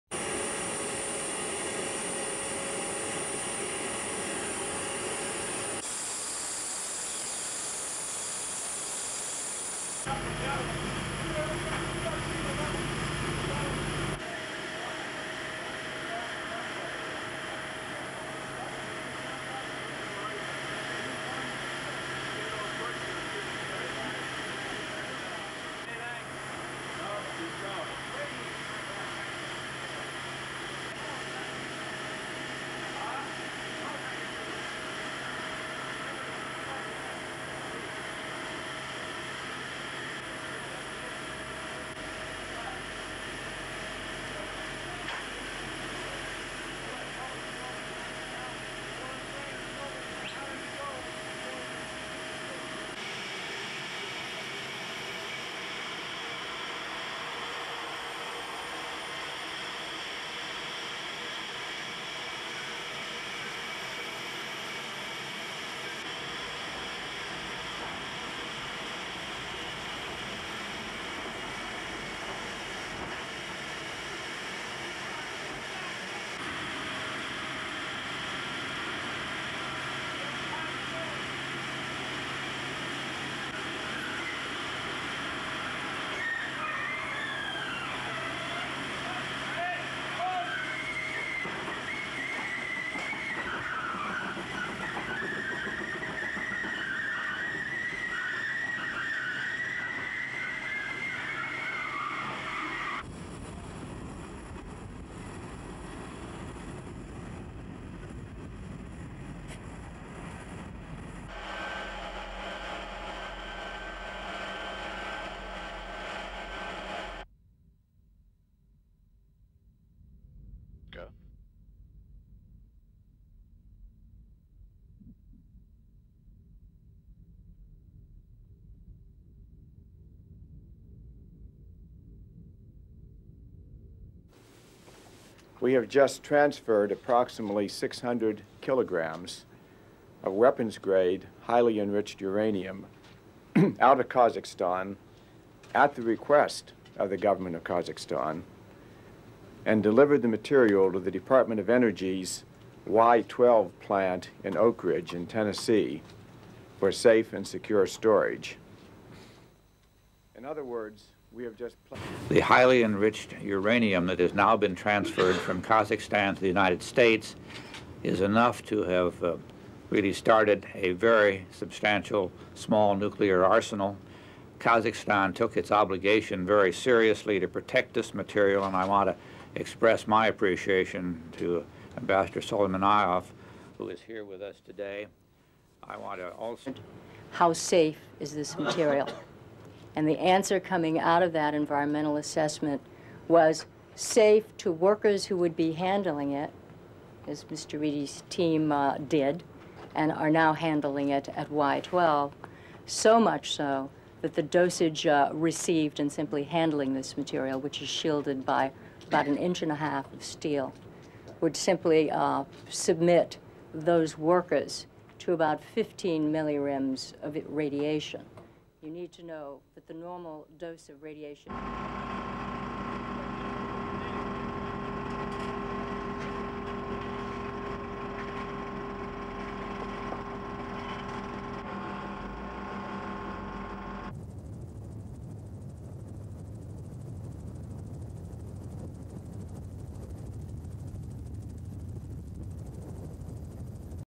USA: NUCLEAR WASTE AIRLIFTED FROM KAZAKHSTAN
(23 Nov 1994) Eng/Nat

The United States has secretly transferred enriched uranium from the former Soviet  republic of Kazakhstan to America.

The three million dollar airlift of the nuclear material was completed on Monday - but the  Pentagon has only now released details.

In the snow covered wastes of Kazakhstan enough nuclear material to start a small  arsenal was loaded on board transport planes.

27 American atomic energy agency experts oversaw the airlift agreed upon by  governments of Kazakhstan, Russia and the US.

The highly enriched uranium could have been turned into about two dozen nuclear  weapons. Few knew of the plan in advance - this was declared top secret. The only  images available were taken by the US team on the ground.

President Clinton personally gave the green light for the mission after a request from  Kazakhstan.

The former Soviet republic is being compensated for the nuclear material though the  exact figure has not been made public.

SOUNDBITE:

We have transferred approximately 600 kilograms of weapons grade highly enriched  uranium out of Kazakhstan at the request of the government of Kazakhstan and  delivered the material to the department of energy twelve plant in Oakridge in  Tennessee for safe and secure storage.

SUPER CAPTION: William Perry, U-S Secretary of Defence

SOUNDBITE:

The highly enriched uranium that has now been transferred from Kazakhstan to the  United States is enough to have started a really substantial small nuclear arsenal.  Kazakhstan took its responsibility very seriously to protect this material.

SUPER CAPTION: Warren Christopher, U-S Secretary of State

SOUNDBITE:

How safe is this material and the answer is coming out of that environmental  assessment was that it was safe to workers who would be handling it as Mr. Reidy's  team did and are now handling Y 12, so much so that the dosage received in simply  handling this material which was shielded about an inch and a half of steel would simply  submit those workers to about 15 mgs of radiation.

SUPER CAPTION: Hazel O'Leary, U-S Energy Secretary

The material is now underground in the security of this nuclear plant at Oakridge in  Tennessee. It will remain there until private companies compete for the contract to turn  the highly enriched uranium into a lower state of enrichment so it can be used to fuel a  commercial reactor.